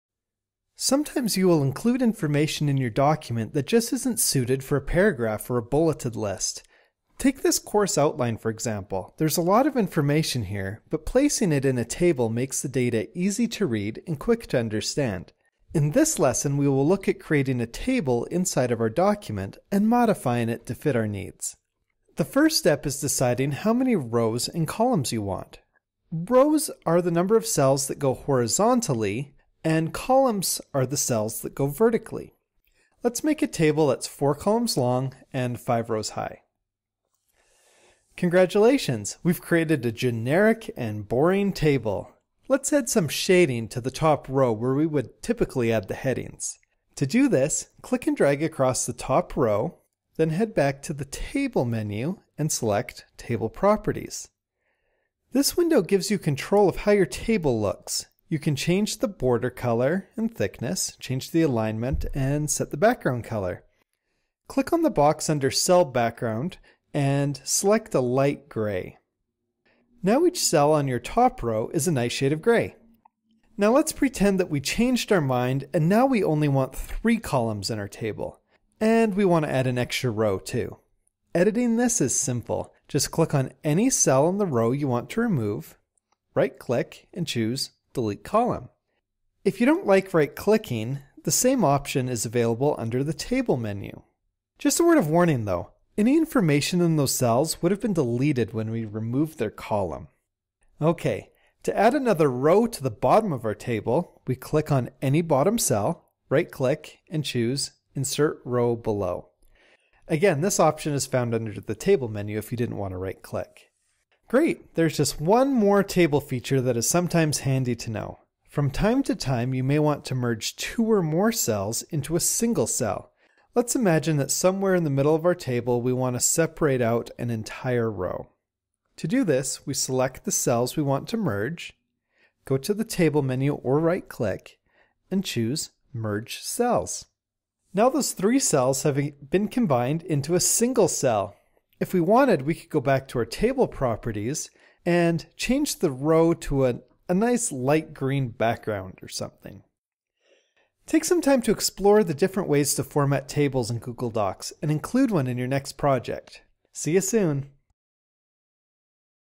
How to add a Table to Google Docs
Sometimes you will include information in your document that just isn’t suited for a paragraph or a bulleted list.

Take this course outline for example.
There is a lot of information here, but placing it in a table makes the data easy to read and quick to understand. In this lesson we will look at creating a table inside a document, and modifying it to fit our needs.

The first step is deciding how many rows and columns you want. Rows, go horizontally and columns go vertically. Let’s make a table that’s 4 columns long and 5 rows high.

Congratulations! We’ve created a generic and boring table. Let’s add some shading to the top row where we would add headings.
To do this, click and drag across the top row, then head back to the Table menu and select Table Properties.

This window gives you control of how your table looks. You can change the border color and thickness, change the alignment, and set a background color. Click on the box under Cell background color and select a light gray.

Now each cell on your top row is a nice shade of gray.

Now let’s pretend that we changed our mind and now only want 3 columns in our table, and want to add an extra row. Editing this is simple. Just click on any cell in the row you want to remove, right click and choose “Delete Column". If you don’t like right clicking the same option is under the Table menu.

Just a word of warning! Any information in those cells would have been deleted when we removed their column.

To add another row to the bottom of our table, we click on any bottom cell, right click and choose “Insert Row Below”. Again, this option is found under the Table menu if you don’t want to right click.

Great! There is just one more table feature that is sometimes handy to know. From time to time you may want to merge two or more cells into a single cell. Let’s imagine that somewhere in the middle of our table we want to separate out an entire row.
To do this we can select all the cells we want to merge, go to the table menu or right click, and choose “Merge Cells”

Now those 3 cells have been combined into a single cell. If we wanted we could go back in to our table properties and add change the row to a light green background.